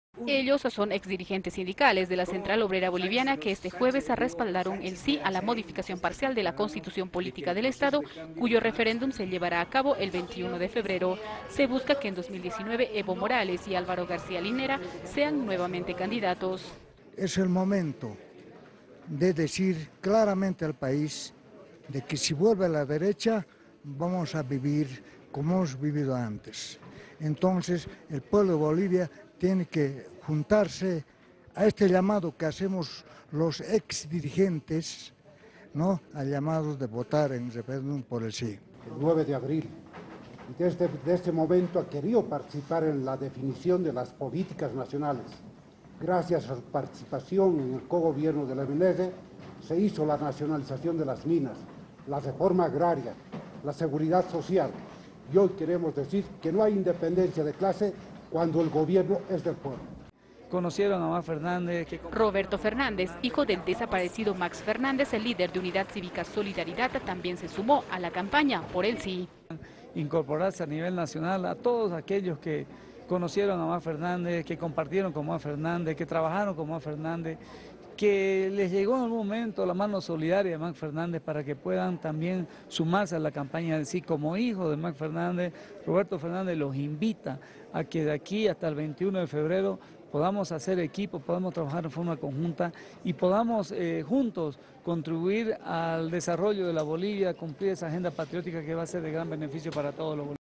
Ex dirigentes sindicales se suman a campaña por el 'Sí' - Jueves 14 de enero 2016
(Abya Yala Digital).- Los ex dirigentes sindicales de la Central Obrera Boliviana este jueves respaldaron la campaña por el 'Sí' para la modificación parcial de la Constitución Política del Estado cuyo referendo se llevará a cabo el 21 de febrero. Entre ellos, Milton Gomez, José Pimentel, y también el político Roberto Fernández. (Video).